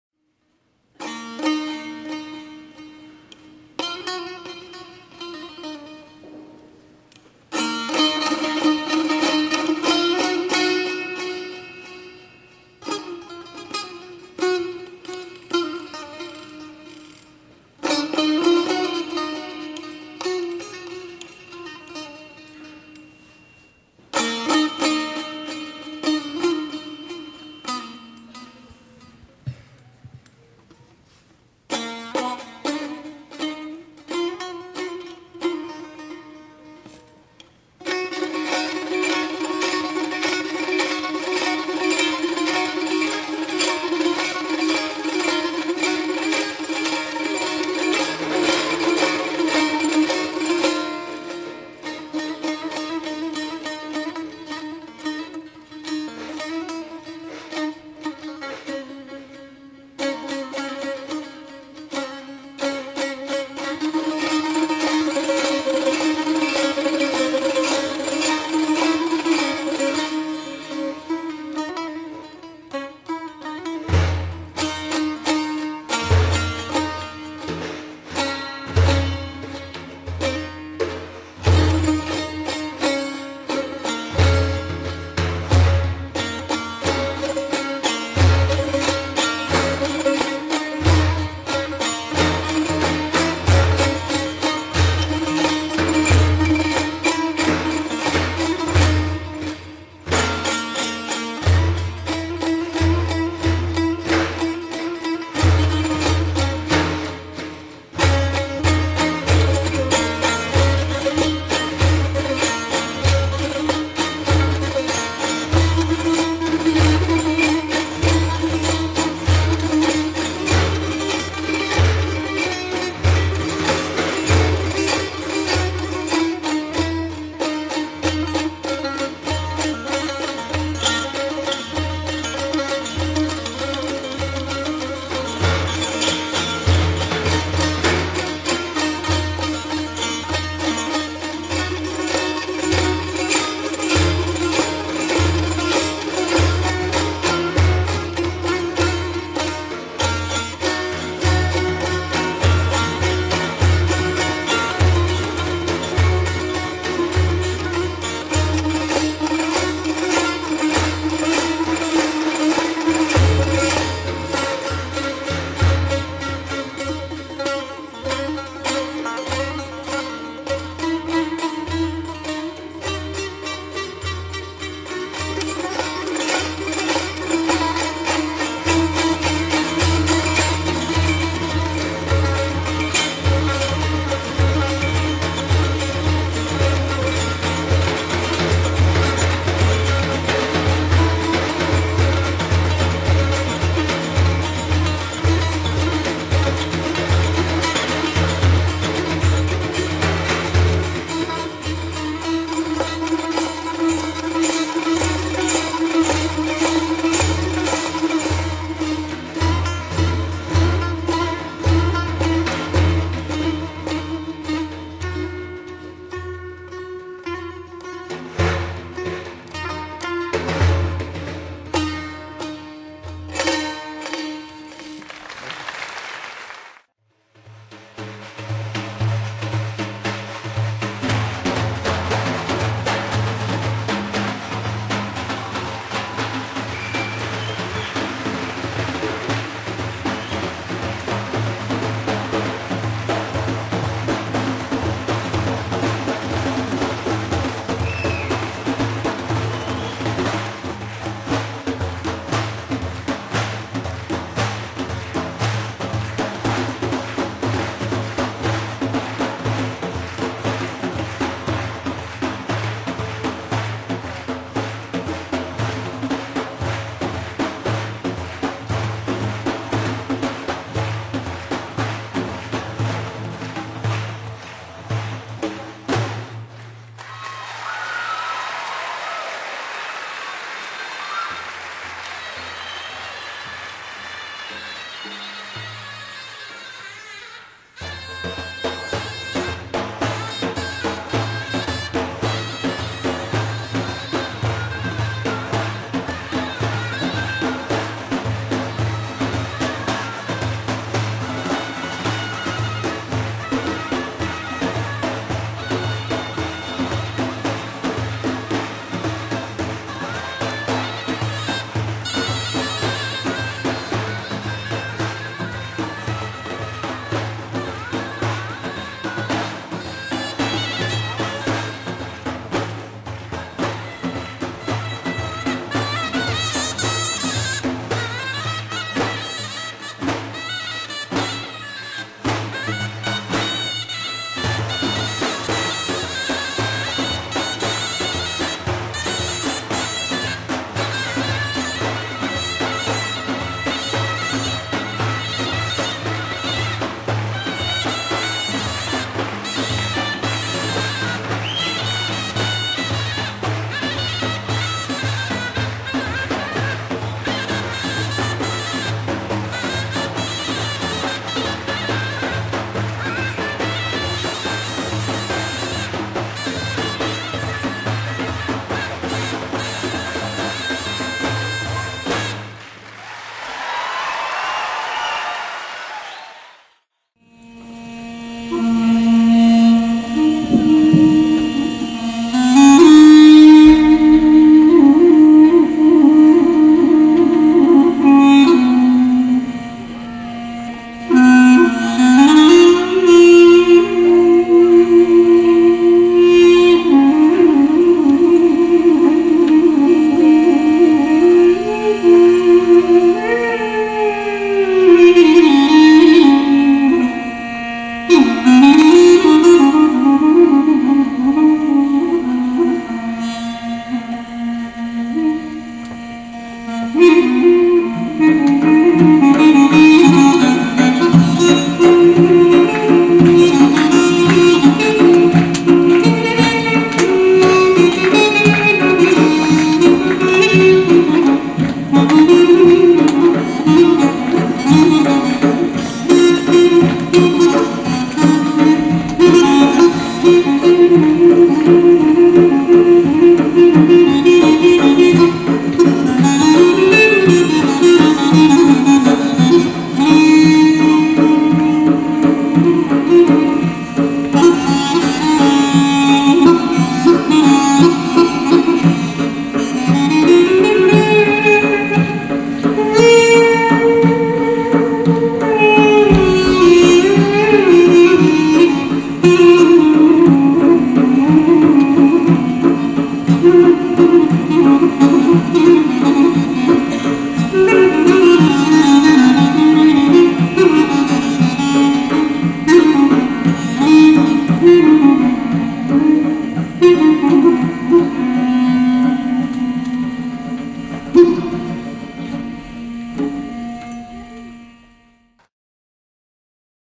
Persian Sufi and regional Music at WorlDance in Salt Lake
Persian Sufi music by Ostad Nejad on tambur and Shirzad Sharif on doira and Persian regional music with 2 doiras and Lloyd Miller on surna plus Miller playing a duduk solo at WorlDance in Salt Lake City